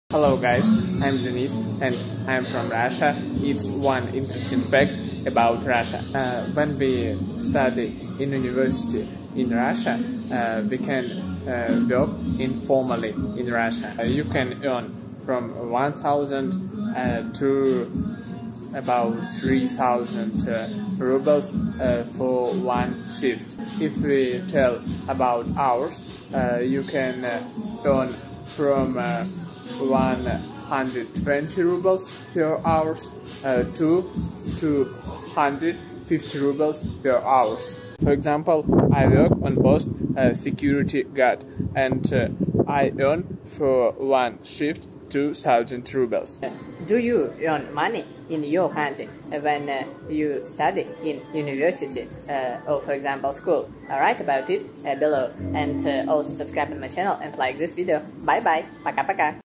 How much students earn in Russia/ Interesting fact about russia/ Russian vlogger/ travel to Russia
Hi everyone!
In this video I want to give you an update of how life in Russia has changed under sanctions 4 months later. I  will show you one interesting fact about Russia.

I wish you all an amazing day!

4) My Binance: 0xc5109617b52a70201abb0935febf7430c6af7ba4